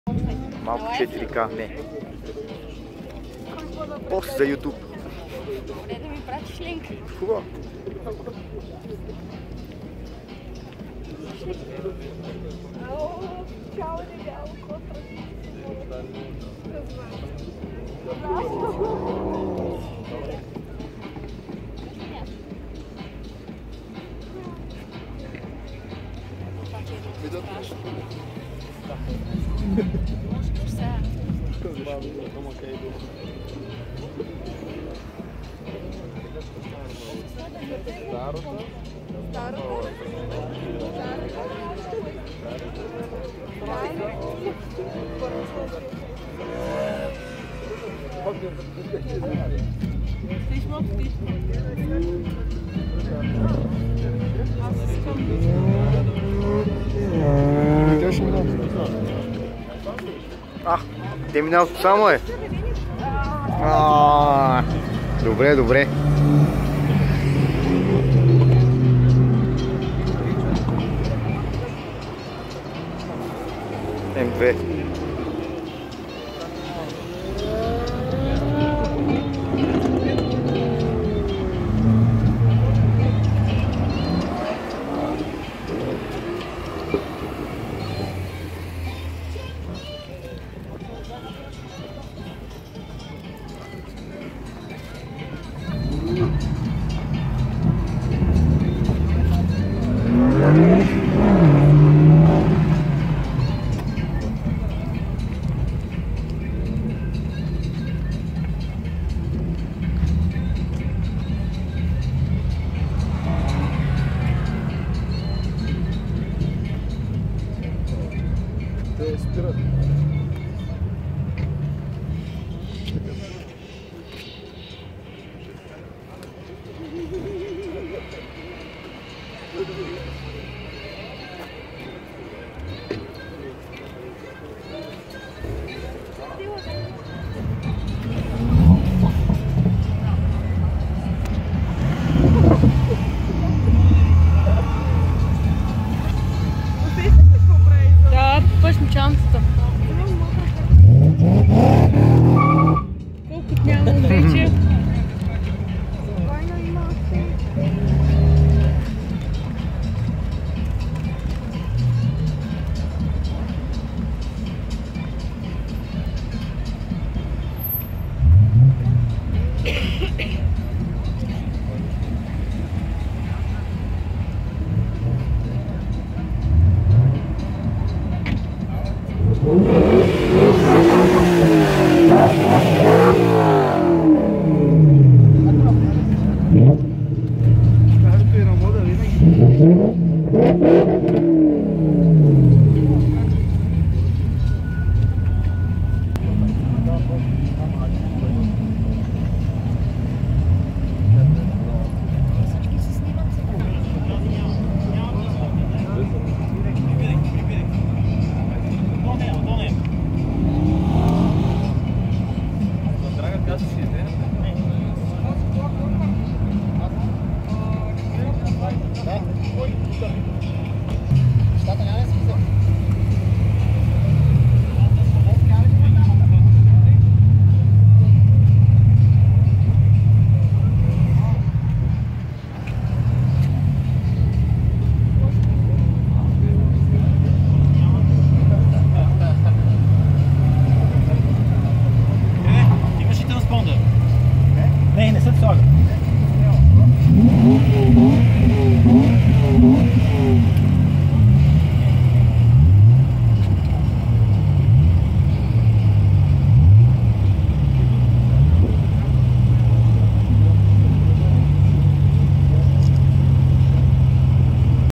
BMW M-WEEKEND 4K BULGARIA 2019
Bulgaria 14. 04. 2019
Track: Dragon, Kaloyanovo, Plovdiv
BMW M1, M2, M3, M4, M5, M6